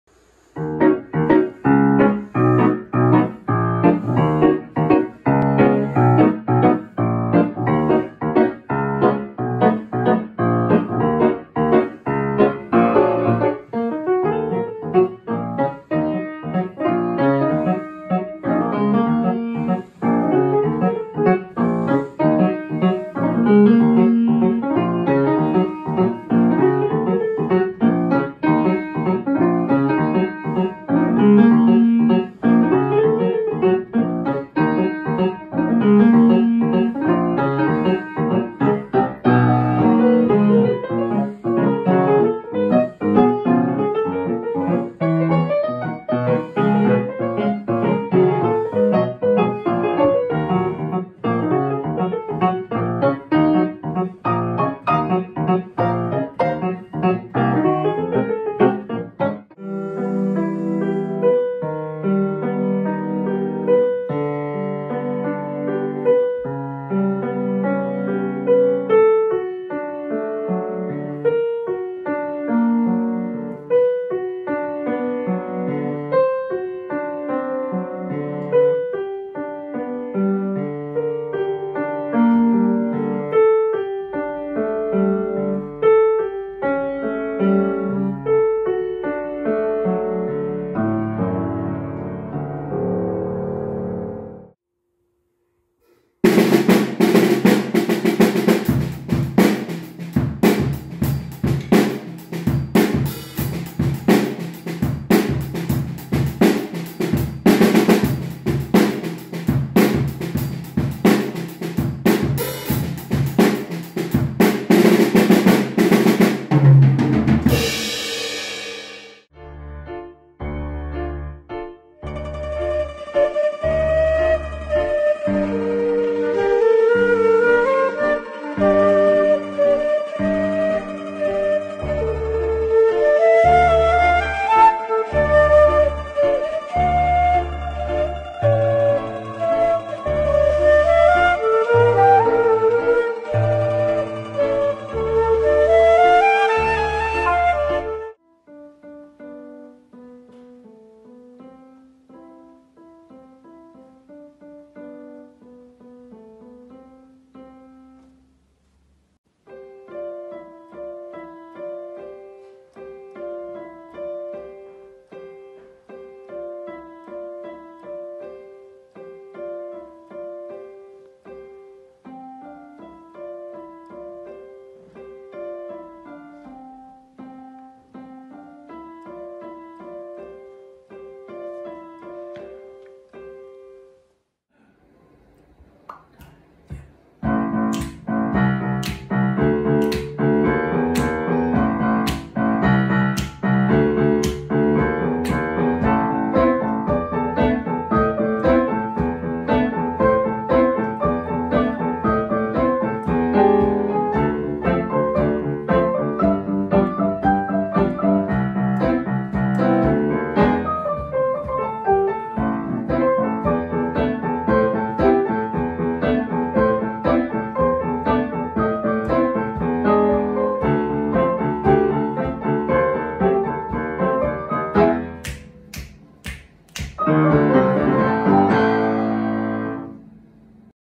(2/2) Clavina feiert Fasching (2 von 2)
Clavina feiert Fasching (2 von 2)
wir verkleiden uns und zeigen uns am Instrument! vielen Dank an alle, die mitgemacht haben, es hat großen Spaß gemacht.